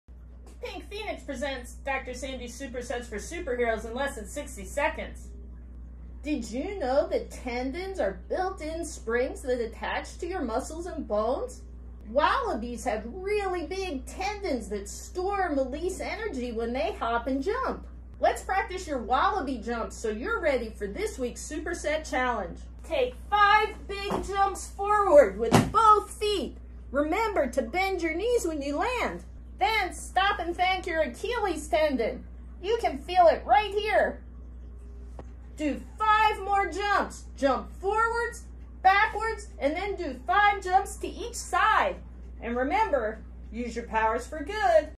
Wallaby Jumps!
Let's do Wallaby Jumps for today's Superset Challenge!

Win a prize! Show us your Beast Moves! Post your picture completing the daily superhero challenge in the comments. One superhero each week will get a prize!

Remember Use Your Powers For Good!